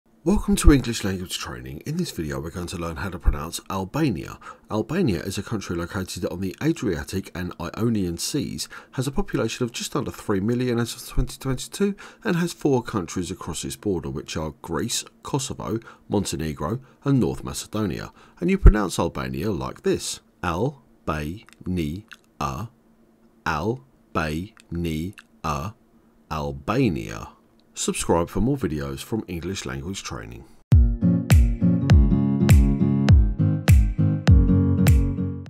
How To Pronounce: Albania (Countries of the World)
Here is a brief overview of and how to pronounce Albania in English as part of our Countries of the World playlist.